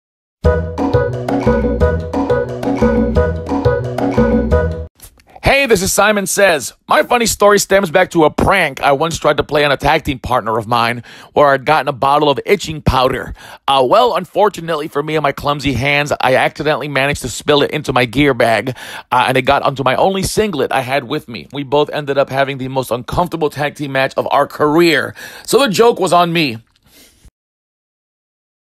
Sounds Sketchy: The Series! episode 2 (feat. Simon Sez)
Welcome to Episode 2 of Sounds Sketchy: The Series! presented by The Milo Beasley Show!

Each episode takes a fun story by our guest narrators, and turns them into a colorful story to be viewed by you! We hope you enjoy this Brand New series, and will be monitoring for future weekly episodes! This week, our pal Simon Sez takes you behind the scenes of a prank gone wrong!

Tell all your friends!